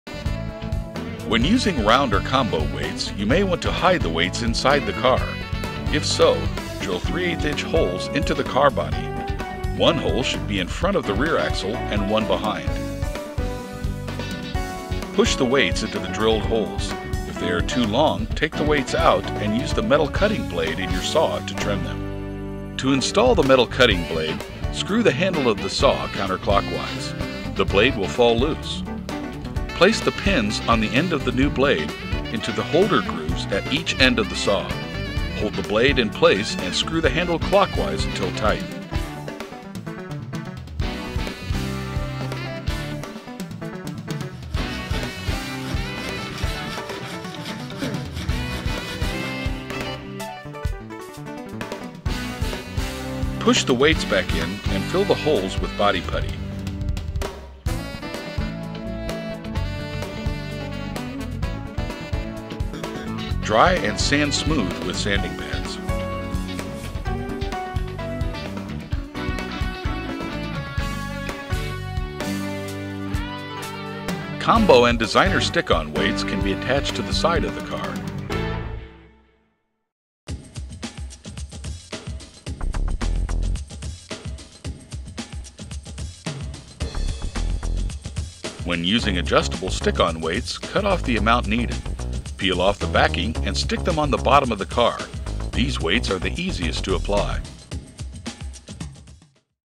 Installing Weights | Pinewood Racing Car | PineCar Derby
- This video demonstrates the different variety of weights that can be used on your derby car.

Items Mentioned in This Video:

Racer Shaping Tools (P3930)
Round Weights (P350)
Combo Weights (P354)
Body Putty (3928)
Designer Stick-On Weights (P379)
Adjustable Stick-On Weights (P378)